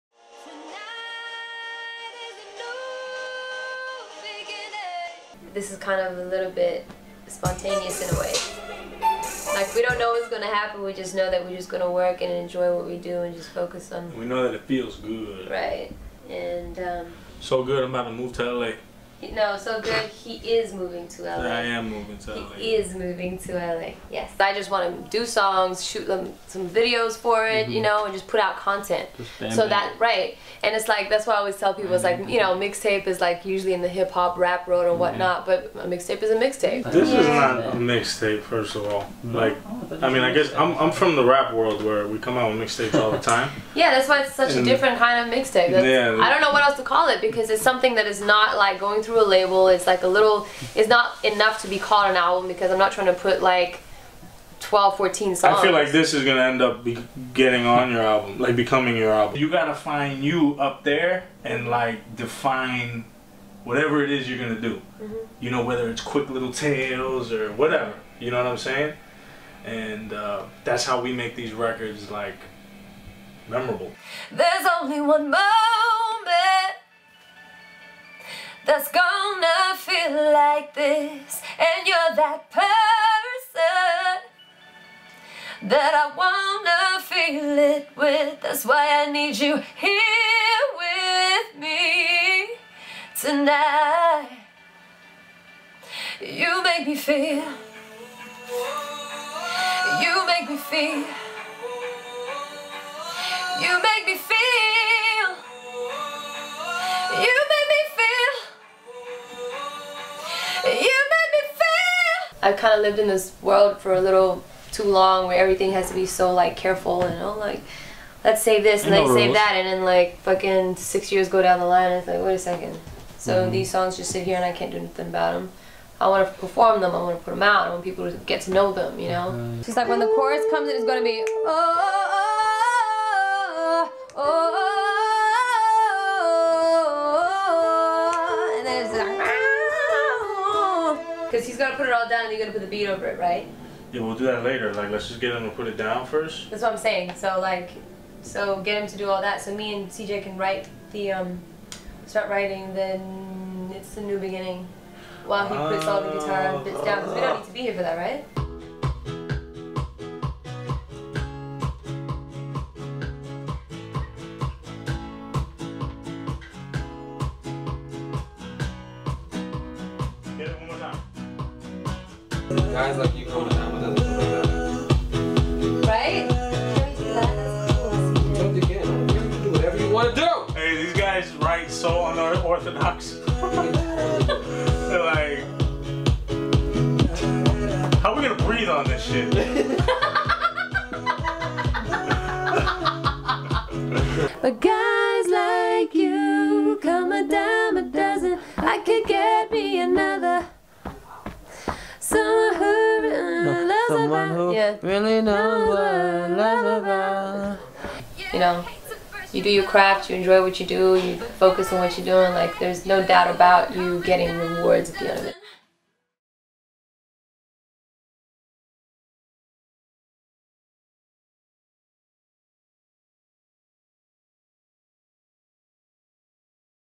Che'Nelle Diaries : Mixtape
I've been working on mixtape for you guys here is some behind the scene footage!!